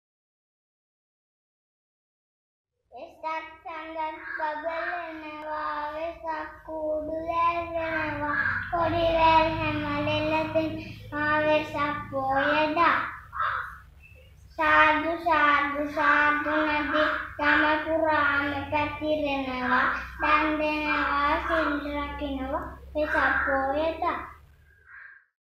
දම්සෝ online වෙසක් කලාපය 2021 | වෙසක් බැති ගී | ගජෙනුල් කමොද් | 1 ශ්‍රේණිය | VESAK BATHI GEE
2021 දම්සෝ online වෙසක් කලාපයට ලැබුණු නිර්මාණ අතරින් අවසන් වටය සදහා තෝරා ගනු ලැබූ නිර්මාණයකි | DAMSO TV | DHARMASOKA COLLEGE | ධර්මාශෝක විද්‍යාලය | දම්සෝ TV |

Welcome to Dharmasoka College Official YouTube Channel